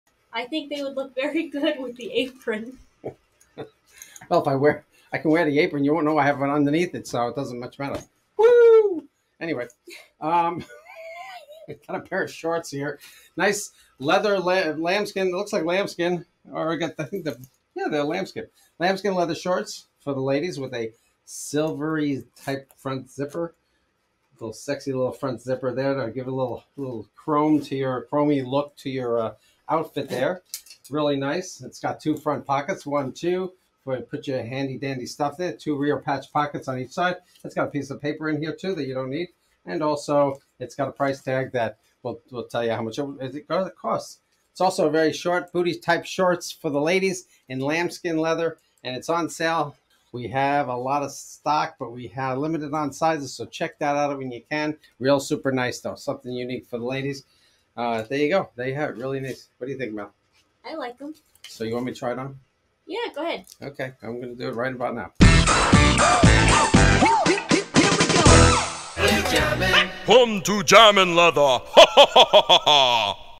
SH1122K - Black Leather Silver Zipper Shorts With Pant Pockets
Black leather booty shorts at a blow out bargain sale price. These cowhide leather shorts come with a shimmering silver and black front zipper. Short shorts with pockets that fits best for women with more narrow hips. A cute looking pair of ladies leather booty shorts with two front pockets, two rear patch pockets and are made of premium quality cowhide leather. Not only are these great looking but they are at a price so cheap you can't pass them up. We recommend measuring before purchasing. Sizes run small